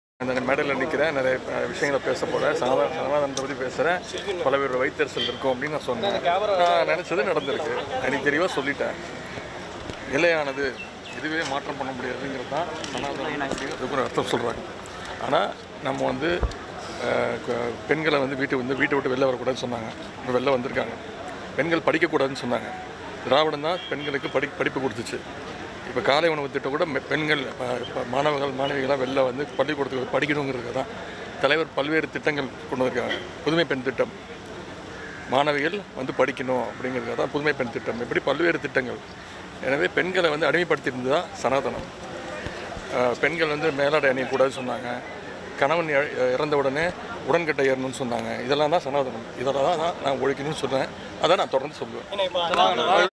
அமைச்சர் உதயநிதி ஸ்டாலின் தூத்துக்குடி விமான நிலையத்தில் பரபரப்பு பேட்டி
தூத்துக்குடி விமான நிலையத்தில் திமுக கழக இளைஞரணி செயலாளரும் தமிழக இளைஞர் நலன் மற்றும் மேம்பாட்டுத்துறை அமைச்சருமான உதயநிதி ஸ்டாலின் பரபரப்பு பேச்சு